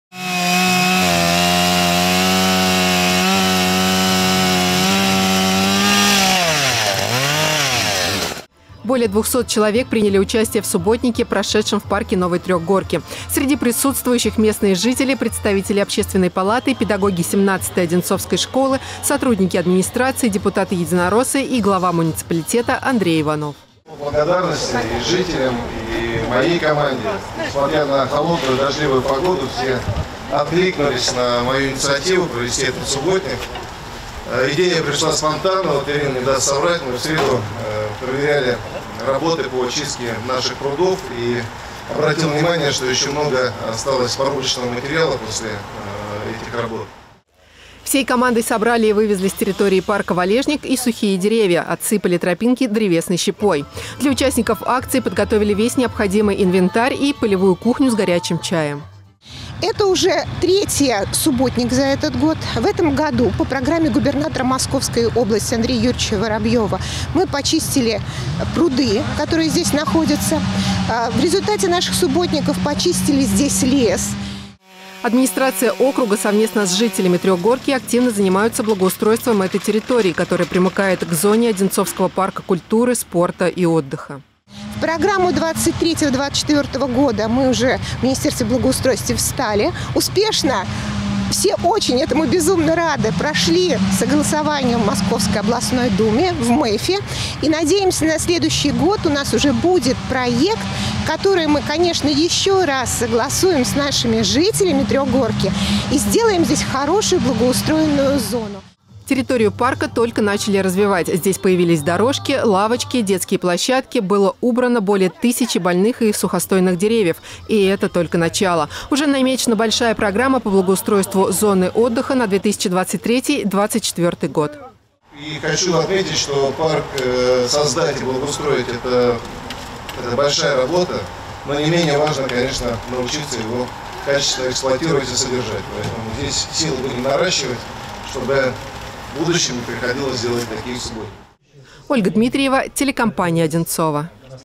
Субботник в Трехгорке
В парке Новой Трёхгорки провели масштабный субботник. Андрей Иванов, местные жители, сотрудники администрации, депутаты, представители Общественной палаты и педагоги Одинцовской школы № 17 – всего более 200 человек убрали сухие деревья и валежник, отсыпали тропинки древесной щепой.